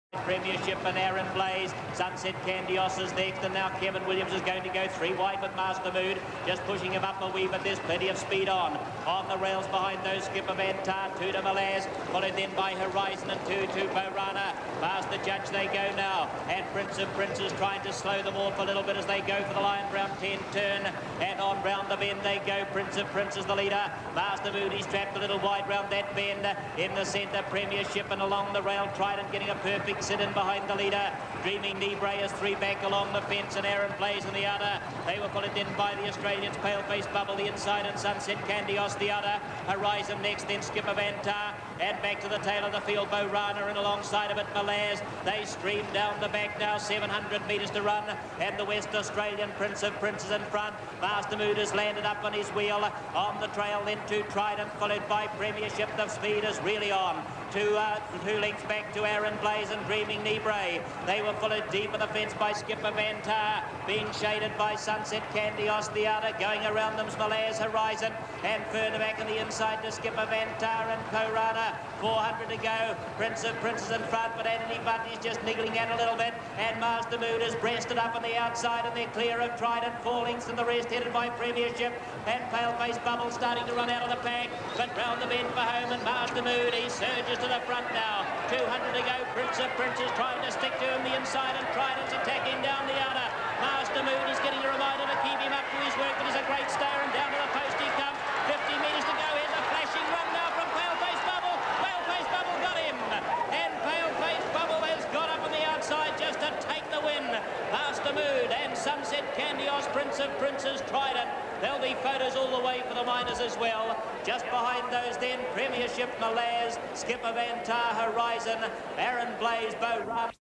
1987 Inter Dominion Heat 7 - Paleface Bubble
Harness Racing from Addington Raceway, Christchurch.